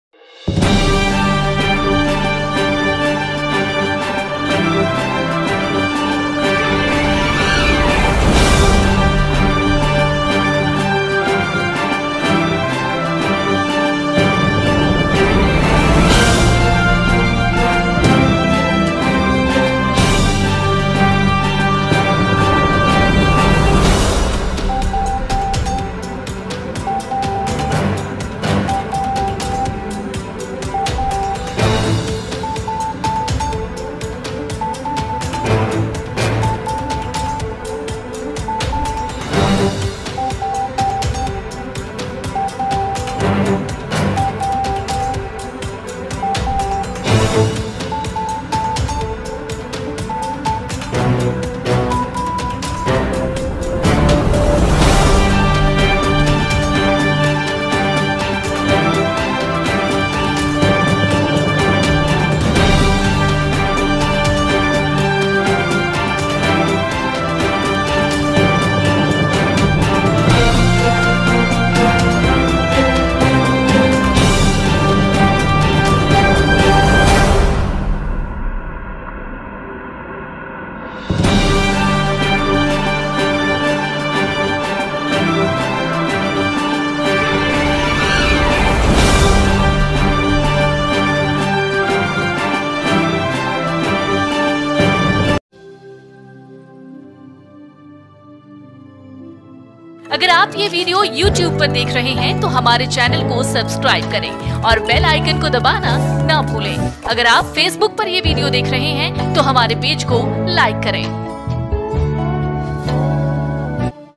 बद्रीनाथ हाईवे पर भूस्खलन का डराने वाला वीडियो
उत्तराखंड में बद्रीनाथ नेशनल हाईवे पर लंगसी के पास हुए भूस्खलन से बद्रीनाथ राष्ट्रीय राजमार्ग अवरुद्ध हो गया हैLandslide